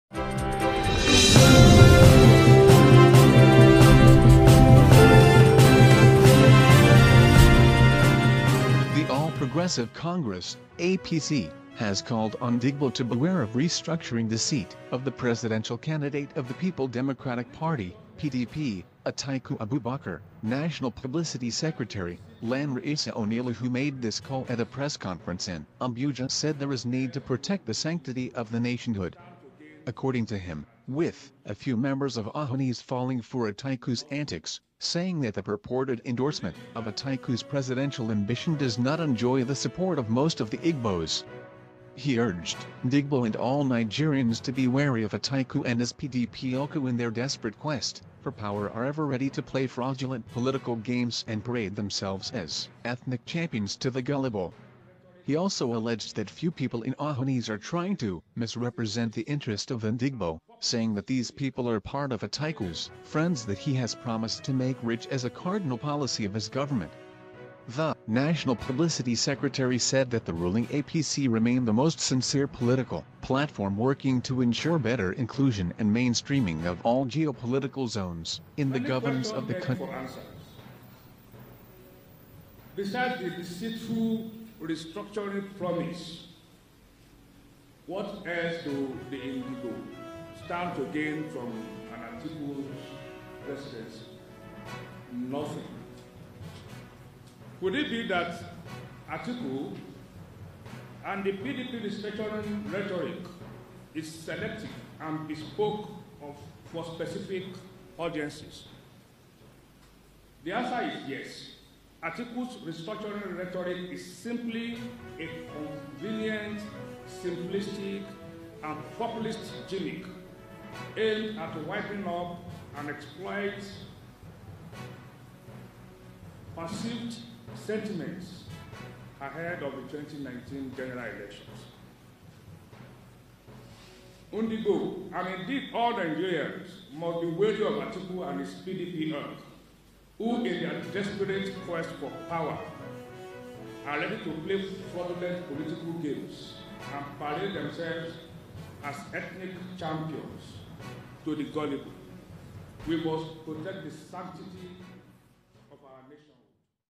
APC WARNS NDIGBO - Beware Of Atiku's Restructuring Promise
The All Progressive Congress, APC, has called on Ndigbo to beware of restructuring deceit of the Presidential Candidate of the People Democratic Party, PDP, Atiku Abubakar.
National Publicity Secretary, Lanre Issa-Onilu who made this call at a press conference in Abuja said there is need to protect the sanctity of the nationhood.
According to him, with a few members of Ohaneze falling for Atiku's antics, saying that the purported endorsement of Atiku’s presidential ambition does not enjoy the support of most of the Igbos.
He urged Ndigbo and all Nigerians to be wary of Atiku and his PDP ilk who in their desperate quest for power are ever ready to play fraudulent political games and parade themselves as ethnic champions to the gullible.
He also alleged that few people in Ohaneze are trying to misrepresent the interest of the Ndigbo, saying that these people are part of Atiku's friends that he has promised to make rich as a cardinal policy of his government.
The National Publicity Secretary said that the ruling APC remained the most sincere political platform working to ensure better inclusion and mainstreaming of all geo- political zones in the governance of the country.